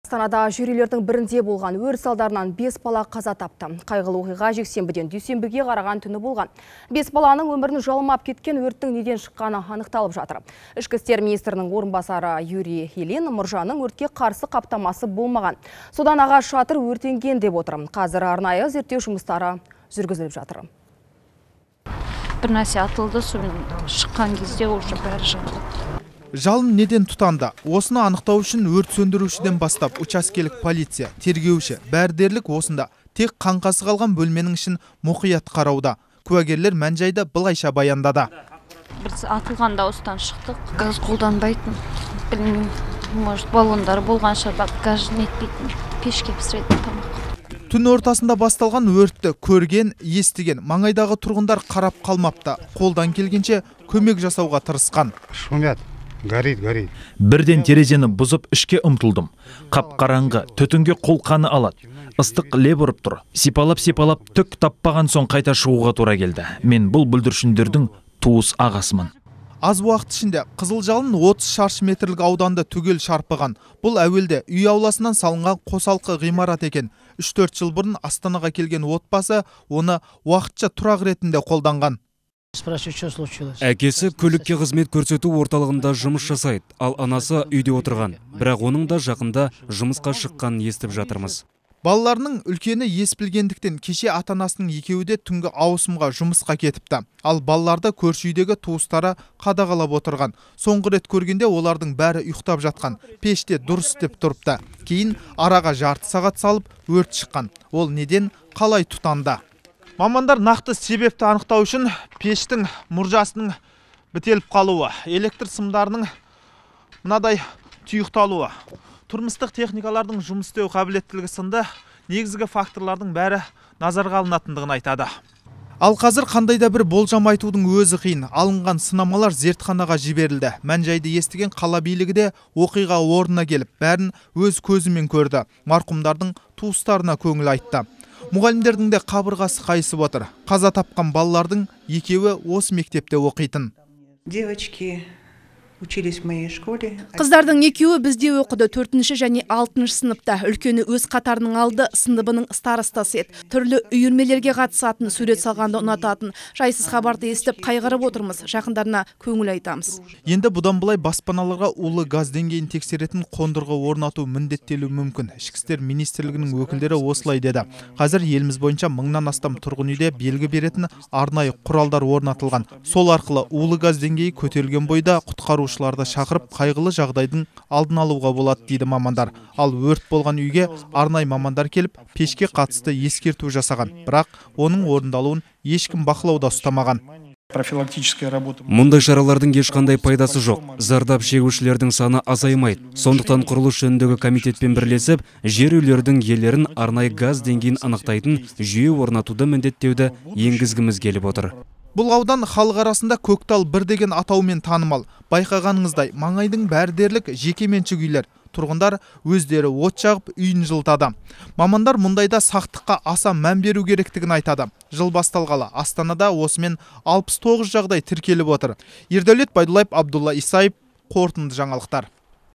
Астанада жер үйлердің бірінде болған өрт салдарынан 5 бала қаза тапты
Қайғылы оқиға жексенбіден дүйсенбіге қараған түні болған.

Бес баланың өмірін жалмап кеткен өрттің неден шыққаны анықталып жатыр. Ішкі істер министрінің орынбасары Юрий Ильин: «Мұржаның өртке қарсы қаптамасы болмаған, содан ағаш шатыр өртенген», – деп отыр. Қазір арнайы зерттеу жұмыстары жүргізіліп жатыр.

Жалын неден тұтанды? Осыны анықтау үшін өрт сөндірушіден бастап учаскелік полицей, тергеуші, бәрі дерлік осында тек қаңқасы қалған бөлменің ішін мұқият қарауда.